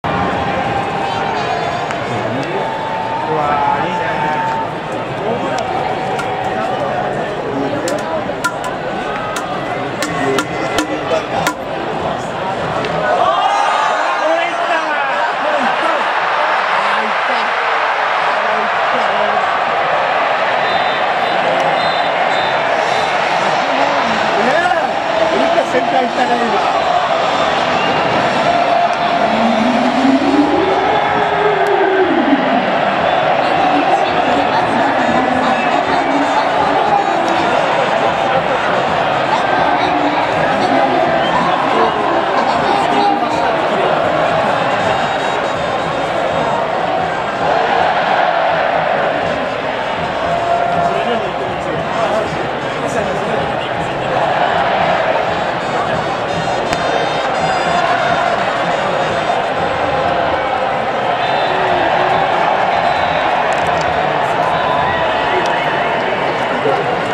2016年5月22日 東京ヤクルトー 横浜DeNA 雄平選手 第３号ソロホームラン
７回裏１アウト走者無し
５番レフト・雄平選手の第３打席
横浜先発・山口投手の初球カーブを
完璧に捉えた会心の一振りは
ライトスタンドへ第３号ソロホームラン